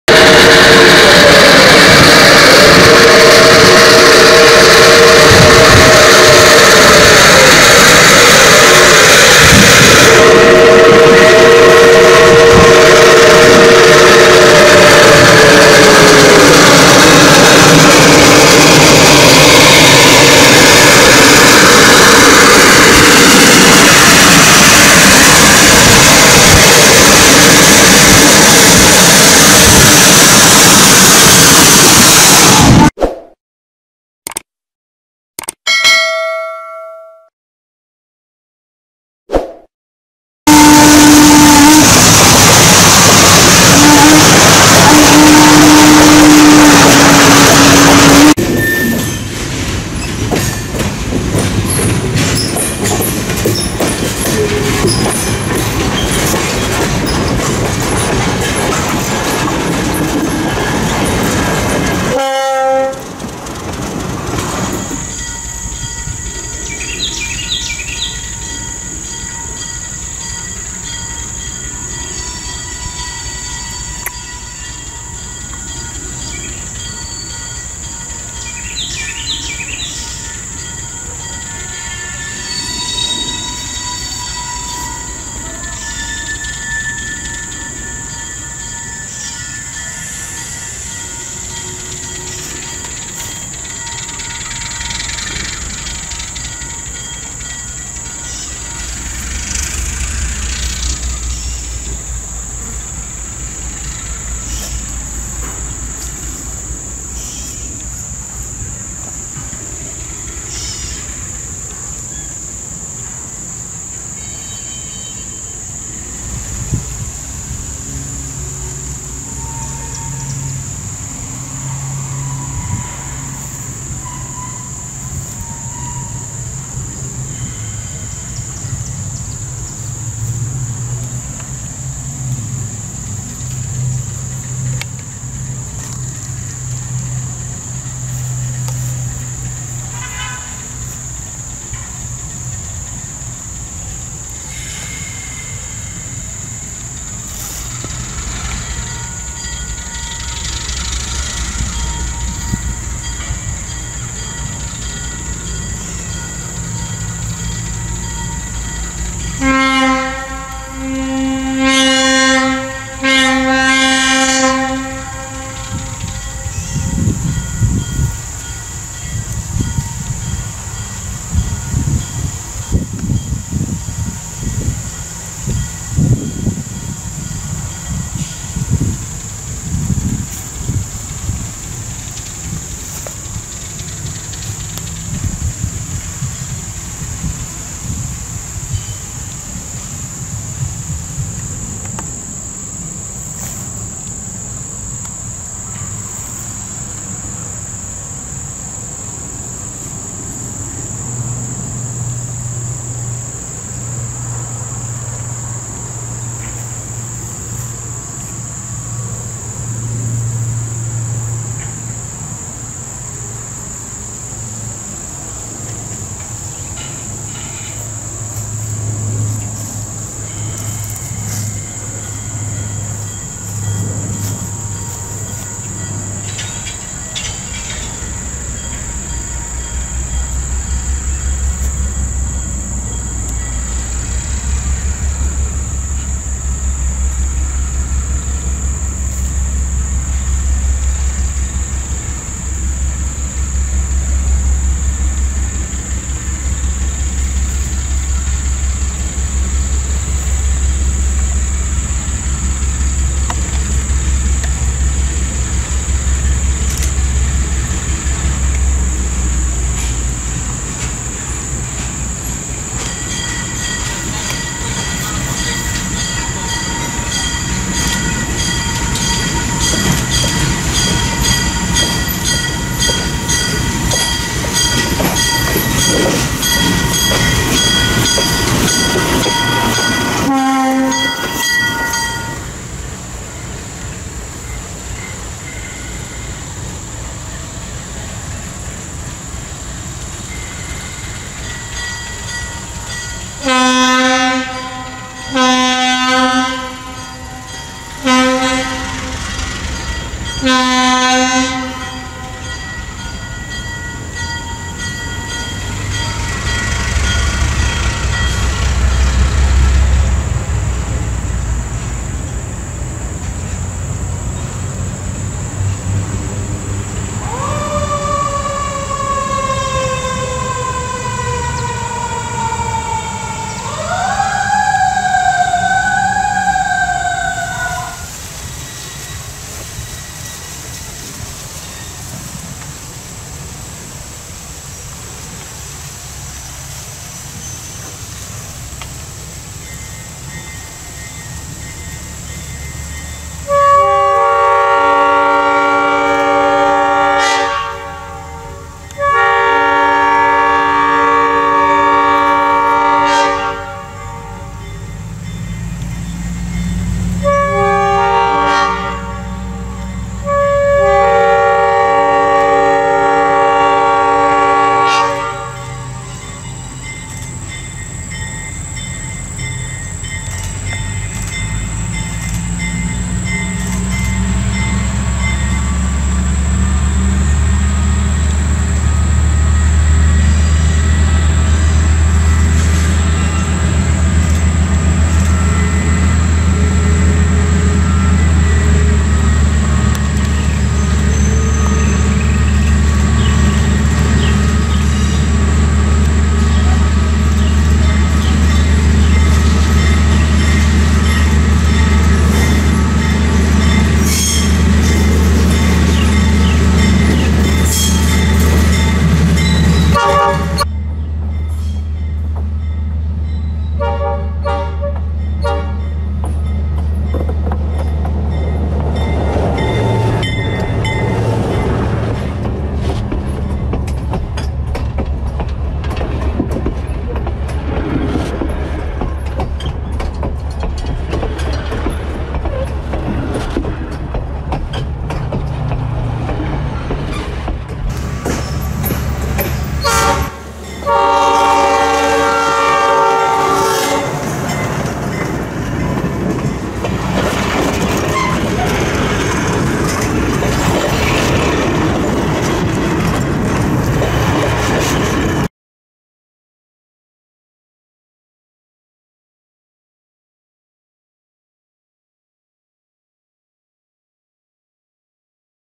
RAILFANNING THE MIDDLETOWN AND HUMEELS TOWN RAILROAD
on this episode of steam enthusiast productions we will... see some freight action as well as a passenger excursion  this was done with JERRYDOESTHINGS